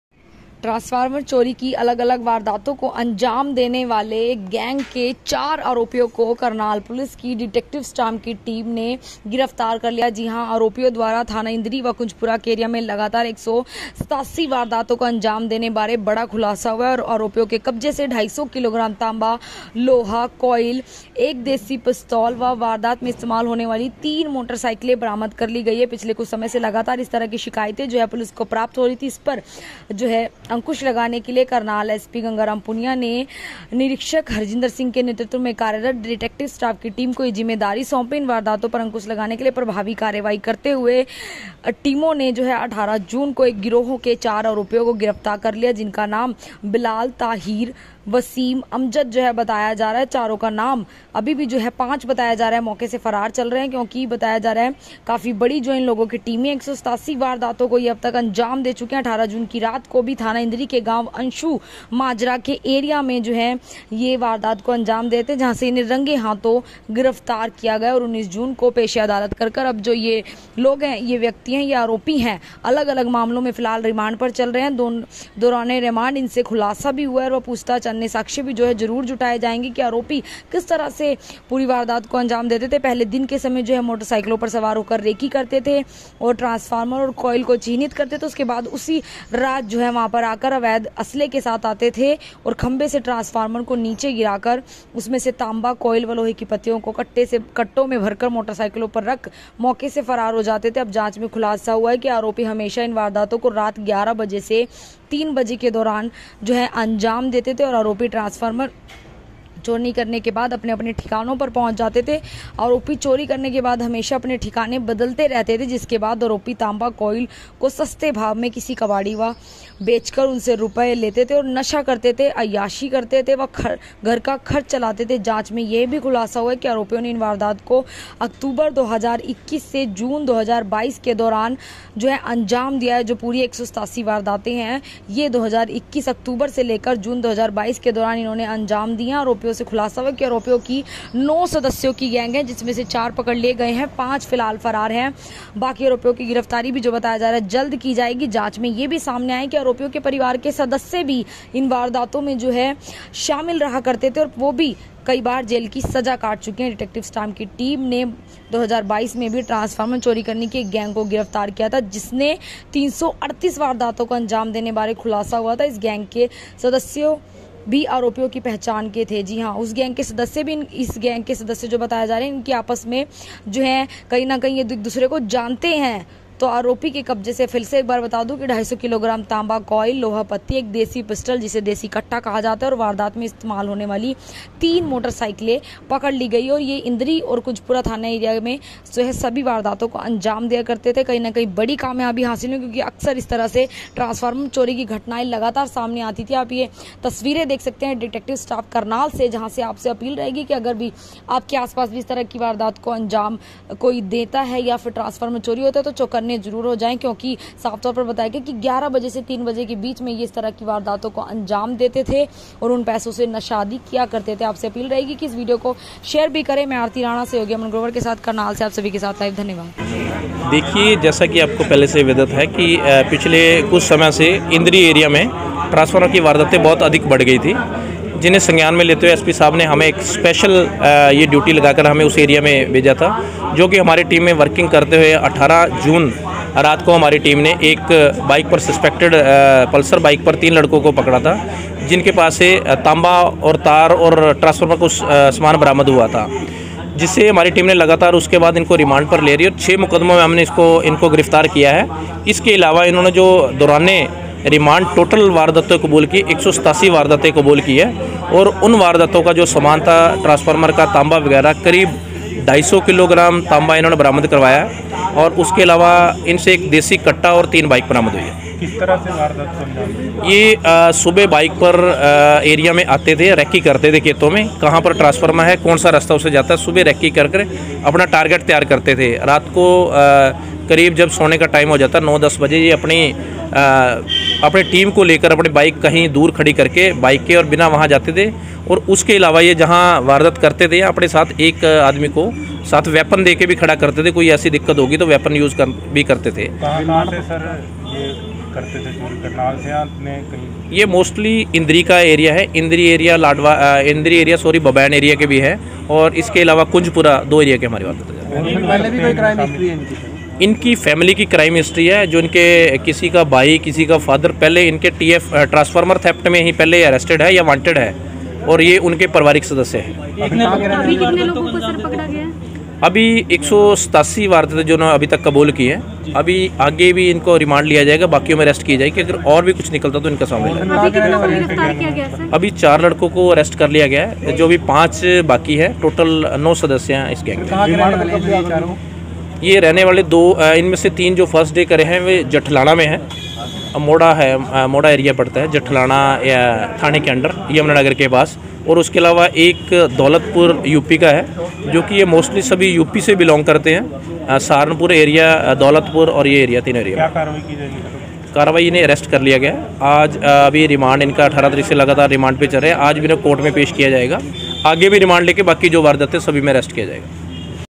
187 चोरी की वारदातों को अंजाम देने वाले 4 चोर गिरफ्तार, देसी कट्टा बरामद, 9 लोगों का है गिरोह 5 फरार
187 चोरी की वारदातों को अंजाम देने वाले 4 चोर गिरफ्तार, देसी कट्टा बरामद, 9 लोगों का है गिरोह 5 फरार

प्रेस नोट

 *ट्रांसफार्मर चोरी की वारदातों को देने वाली गैंग के चार आरोपी करनाल पुलिस की डिटेक्टिव स्टाफ टीम ने किए गिरफ्तार,*

 *आरोपियों द्वारा थाना इन्द्री व कुंजपुरा के एरिया की 187 वारदातों को अंजाम देने बारे हुआ खुलासा,*

 *आरोपियों के कब्जे से 250 किलोग्राम तांबा क्वाइल व लोहा पत्ती, एक देशी पिस्तौल व वारदात में इस्तेमाल तीन मोटरसाईकिल की गई बरामद,*

दिनांक 04 जुलाई 2022 करनाल,                 जिला पुलिस करनाल को पिछले कुछ समय से लगातार ट्रांसफार्मर चोरी होने की शिकायतें प्राप्त हो रही थी। इन वारदातों पर अंकुश लगाने के लिए पुलिस अधीक्षक करनाल श्री गंगाराम पुनिया ने निरीक्षक हरजिन्द्र सिंह के नेतृत्व में कार्यरत डिटेक्टिव स्टाफ की टीम को जिम्मेदारी सौंपी। इन वारदातों पर अंकुश लगाने के लिए प्रभावी कार्यवाही करते हुए एएसआई देवेन्द्र सिंह व एएसआई गुरमीत सिंह
की अध्यक्षता में टीमों द्वारा दिनांक 18 जून 2022 को एक गिरोह के चार आरोपियों को गिरफ्तार करने में कायमाबी हासिल की है। *टीम द्वारा आरोपी 1. बिलाल पुत्र सहजादीन वासी गांव मौडी जठलाना जिला यमुनानगर 2. ताहिर पुत्र तहसीन वासी गांव शांहजापुर थाना किटोर जिला मेरठ उत्तर प्रदेश 3. वसीम अकरम पुत्र मोहम्मद अली वासी दौलतपुर थाना गंगोह जिला सहारनपुर उत्तर प्रदेश व 4. अमजद उर्फ बग्गी पुत्र इरफान वासी छोटा बांस थाना रादौर हाल मौडी जठलाना जिला यमुनानगर* को दिनांक 18 जून की रात को थाना इन्द्री के गांव अंशू माजरा के एरिया से चोरी की वारदात के अंजाम देते हुए रंगे हाथों गिरफ्तार किया गया। आरोपियों को दिनांक 19 जून को पेश अदालत किया गया और आरोपी फिलहाल विभिन्न मामलों में पुलिस रिमाण्ड पर चल रहे थे। आरोपियों से थाना इन्द्री व कुंजपुरा के एरिया से 187 वारदातों को अंजाम देने बारे खुलासा हुआ है।

   दौराने रिमाण्ड आरोपियों से पूछताछ व अन्य विश्वसनीय साक्ष्यों के आधार पर जांच में खुलासा हुआ कि आरोपी ट्रांसफार्मर चोरी की वारदातों को अंजाम देने से पहले दिन के समय मोटरसाईकिल पर सवार होकर रैकी करते थे। आरोपी रैकी करने के दौरान तांबा क्वाइल वाले ट्रांसफार्मरों को ही चिन्हित करते थे। जिसके बाद आरोपी रात के समय अवैध असले के साथ आते थे और खम्बे से ट्रांसफार्मर को नीचे गिराकर उसमें से तांबा क्वाइल व लोहे की पत्तियों को कट्टों में भरकर मोटरसाईकिलों पर रख मौका से फरार हो जाते थे। जांच में खुलासा हुआ कि आरोपी हमेशा इन वारदातों को रात ग्यारह बजे से तीन बजे के दौरान अंजाम देते थे। आरोपी ट्रांसफार्मर चोरी करने के बाद अपने-अपने ठिकानों पर पंहुच जाते थे। आरोपी चोरी करने के बाद हमेशा अपने ठिकाने बदलते भी रहते थे। जिसके बाद आरोपी तांबा क्वाइल को सस्ते भाव में किसी कबाडी को बेचकर उन रूप्यों से नशा करते, अय्याशी करते व अपने घर का खर्च चलाते थे। जांच में यह भी खुलासा हुआ कि आरोपियों ने इन वारदातों को अक्टूबर 2021 से जून 2022 के दौराने अंजाम दिया था। आरोपियों से खुलासा हुआ कि आरोपियों की नौ सदस्यों की गैंग है। जिसमें से पांच आरोपी अभी फरार चल रहे हैं। बाकि आरोपियों की गिरफ्तारी के भी प्रयास जारी हैं। जांच में यह भी सामने आया कि आरोपियोें के परिवार के सदस्य भी इन वारदातों को अंजाम देते थे और वह भी जेल में कई बार सजा काट चुके हैं। डिटेक्टिव स्टाफ की टीम ने मार्च 2022 में भी ट्रांसफार्मर चोरी करने वाले एक गैंग को गिरफ्तार किया था। जिनसे 338 वारदातों को अंजाम देने बारे खुलासा हुआ था। उस गैंग के सदस्य भी आरोपियों की पहचान के थे।

    *आरोपियों के कब्जे से कब्जे से 250 किलोग्राम तांबा क्वाइल व लोहा पत्ती, एक देशी पिस्तौल व वारदात में इस्तेमाल तीन मोटरसाईकिल बरामद की गई हैं।* आरोपियों को आज पेश अदालत किया जाकर न्यायिक हिरासत में भेजा गया है।